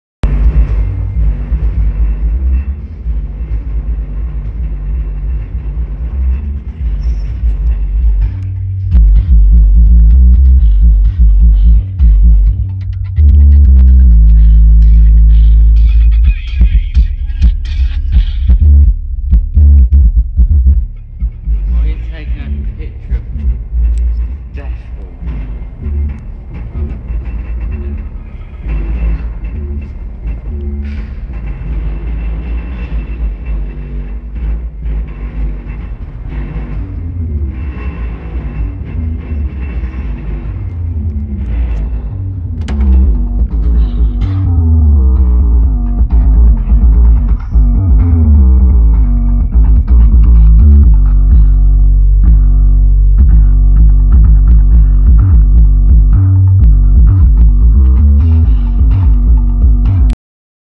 vibe slick subwoofer
short video of my vibe slick sub running half volume and half bass